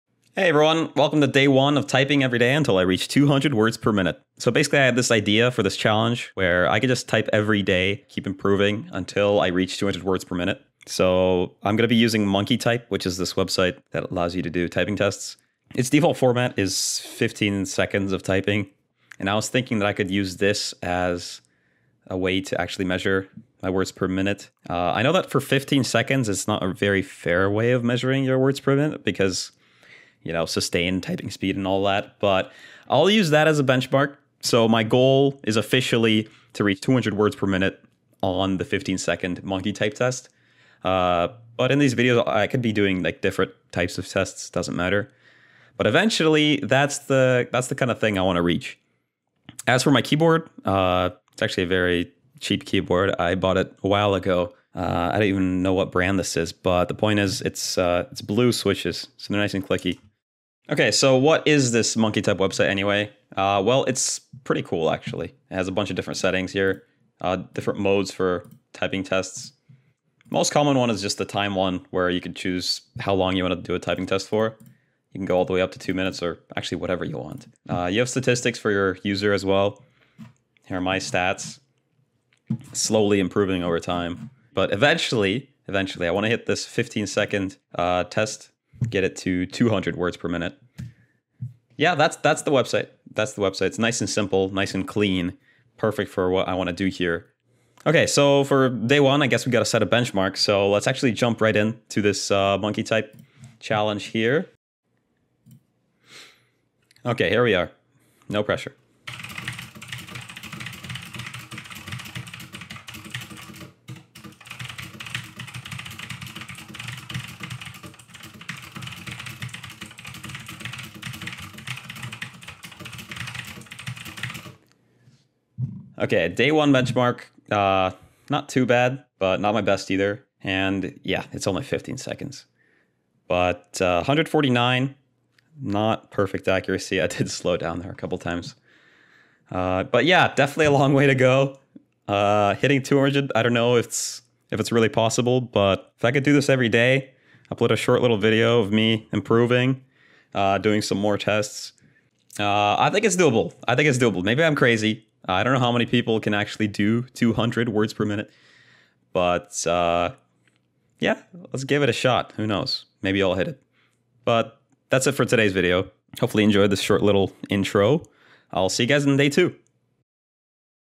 Typing Every Day Until I Reach 200WPM - Day 1
Today, a journey begins. I'm improving my typing speed on MonkeyType until I can hit 200 WPM.
Is this even possible? Come along for the ride to find out!